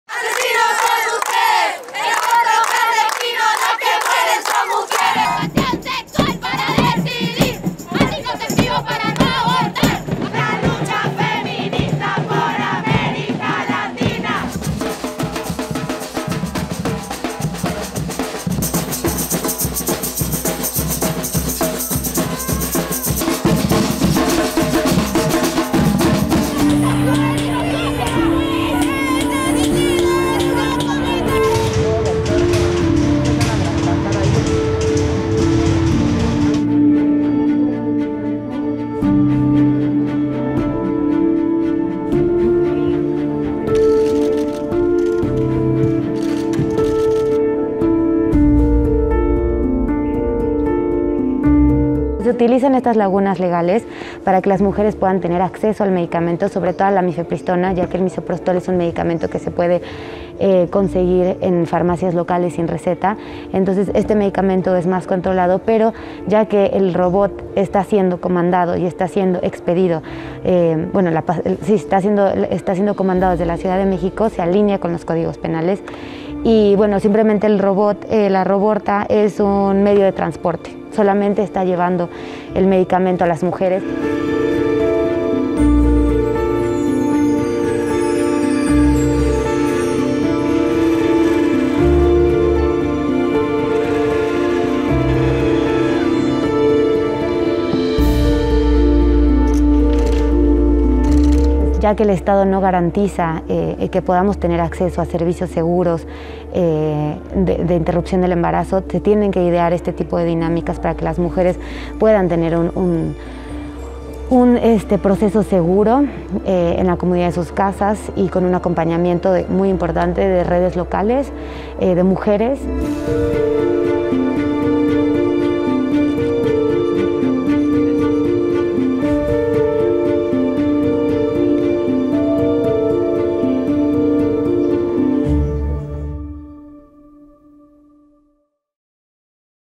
Abortos más seguros gracias a robots
¿Un robot que dispensa píldoras abortivas? Con "rAborta", casi un centenar de asociaciones feministas mexicanas ayudan a las mujeres a interrumpir los embarazos no deseados, de manera segura y sin infringir la ley.